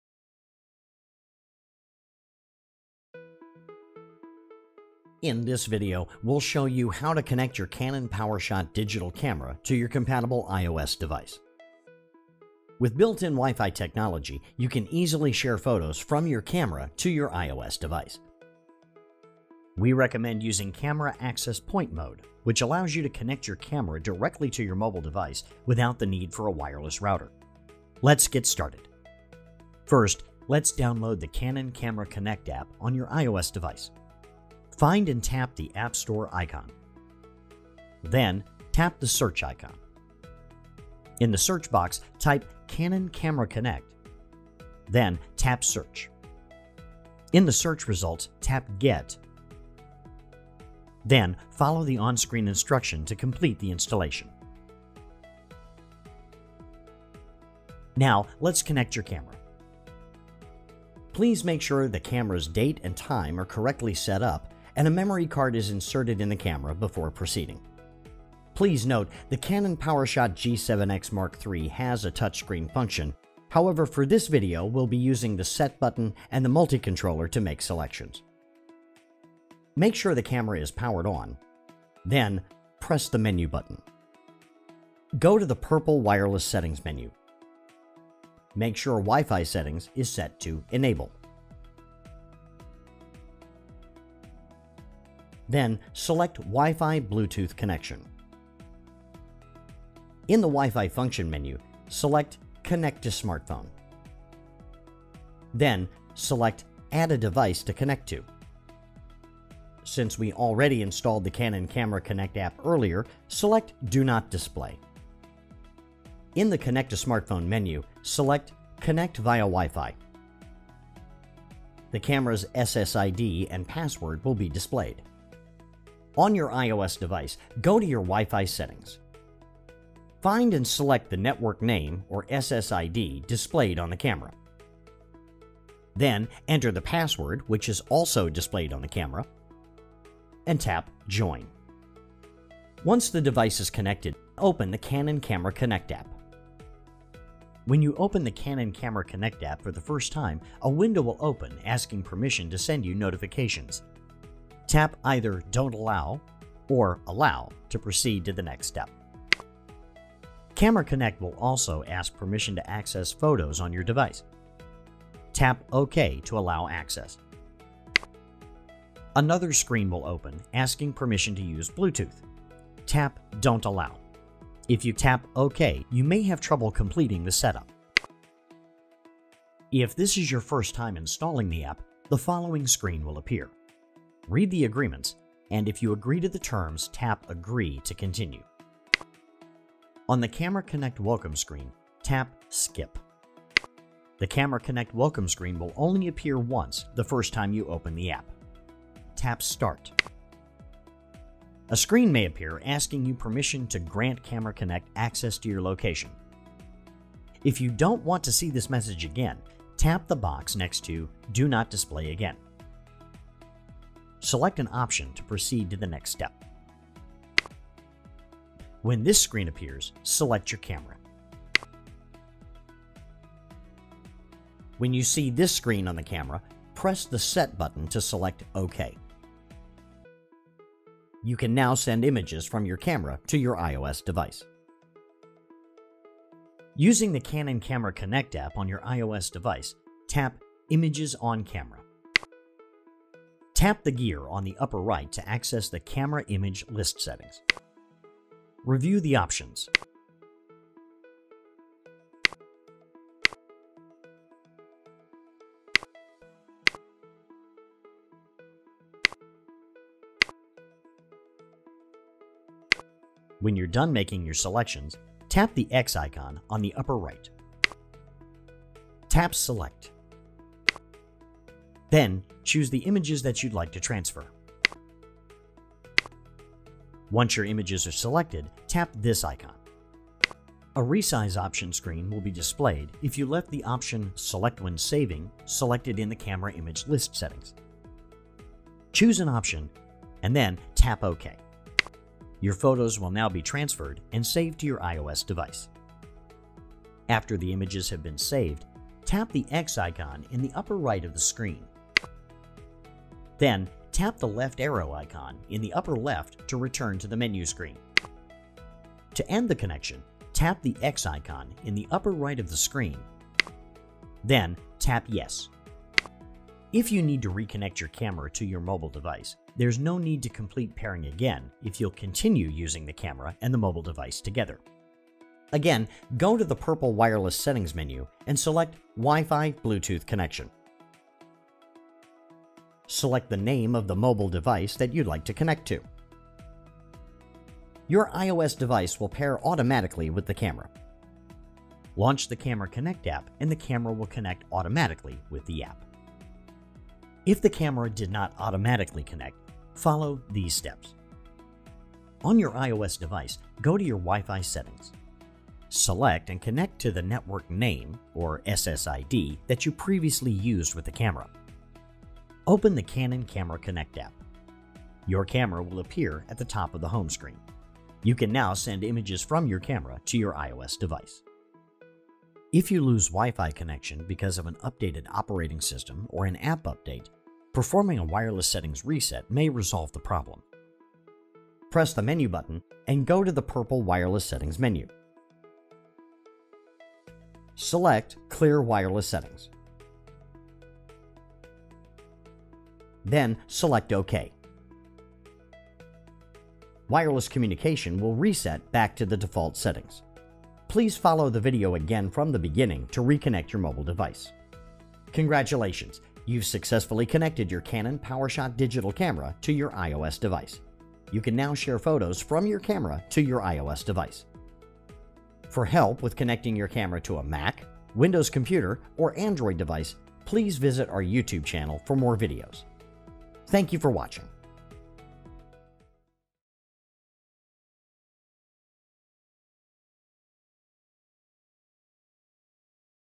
Canon PowerShot G7 X Mark III - Wireless Connection with an iOS Device
Connect your Canon Canon PowerShot G7 X Mark III digital camera to share photos on a wireless network with your compatible iOS device.